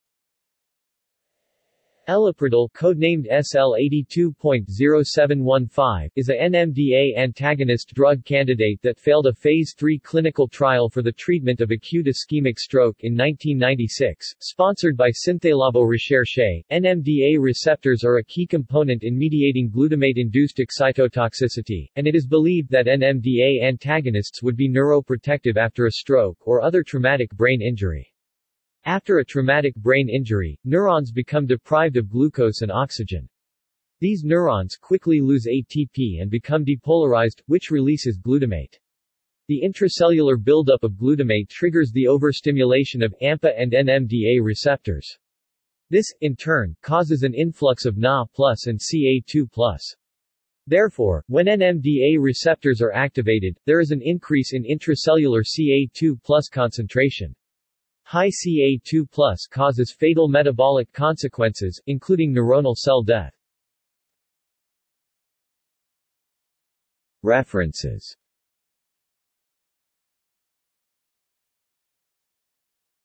Eliprodil | Wikipedia audio article
This is an audio version of the Wikipedia Article:
Eliprodil

SUMMARY
Eliprodil (codenamed SL-82.0715) is a NMDA antagonist drug candidate that failed a Phase III clinical trial for the treatment of acute ischemic stroke in 1996, sponsored by Synthélabo Recherche.NMDA receptors are a key component in mediating glutamate-induced excitotoxicity, and it is believed that NMDA antagonists would be neuroprotective after a stroke or other traumatic brain injury. After a traumatic brain injury, neurons become deprived of glucose and oxygen. These neurons quickly lose ATP and become depolarized, which releases glutamate. The intracellular buildup of glutamate triggers the overstimulation of AMPA and NMDA receptors. This, in turn, causes an influx of Na+ and Ca2+. Therefore, when NMDA receptors are activated, there is an increase in intracellular Ca2+ concentration. High Ca2+ causes fatal metabolic consequences, including neuronal cell death.